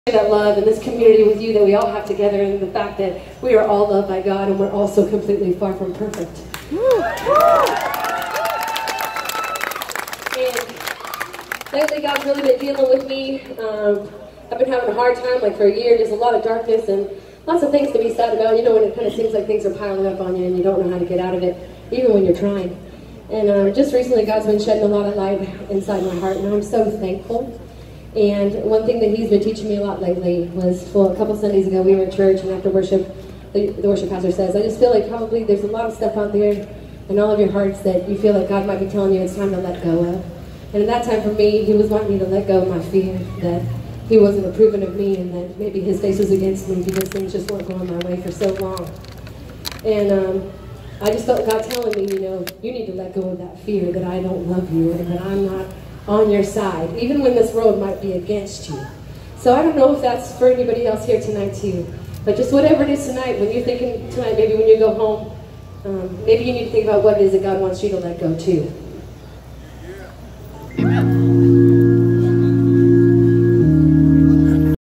Dawn Michelle of Fireflight speaking on fear
This was an introduction by Dawn Michelle of Fireflight. She talked about fear and her moving forward with a worship album that she always feared would never happen.